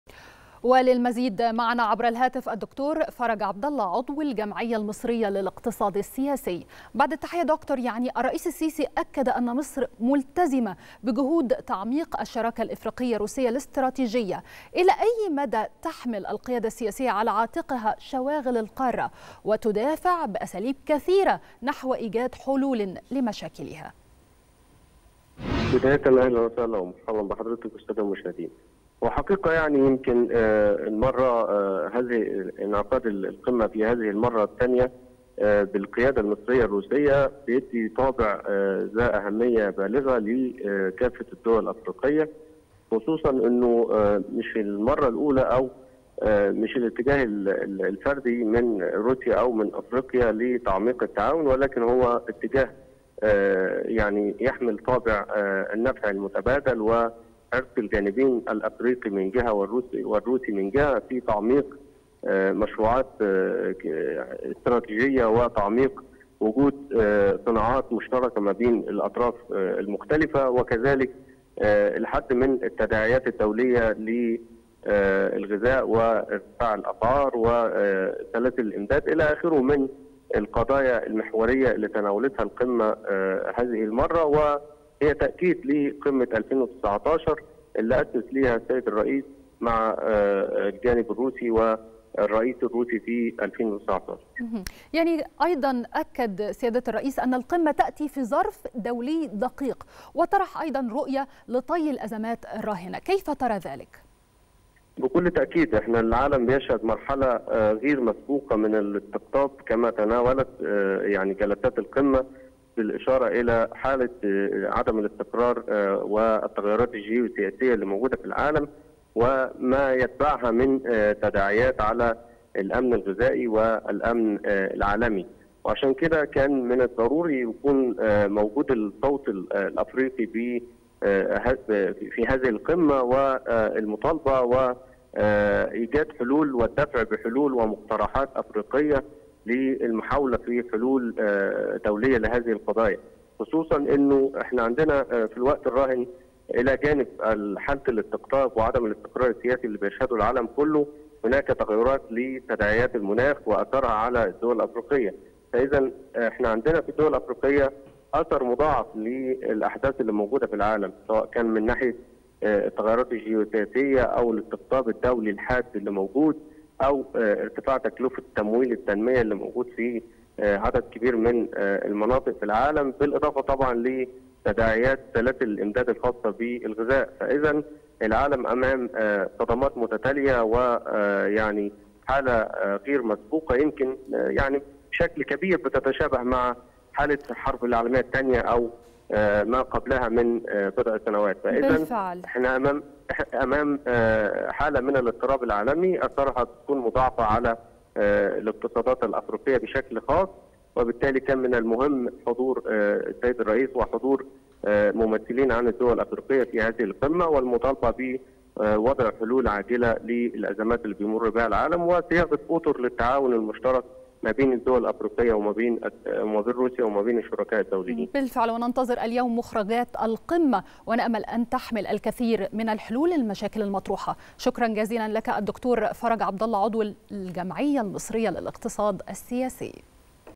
د. فرج عبدالله .. توضح أهم النقاط التي ذكرها الرئيس السيسي خلال خطابه في القمة الروسية الأفريقية
نشرة السادسة | د. فرج عبدالله  ..  توضح أهم النقاط التي  ذكرها الرئيس السيسي خلال خطابه في القمة الروسية الأفريقية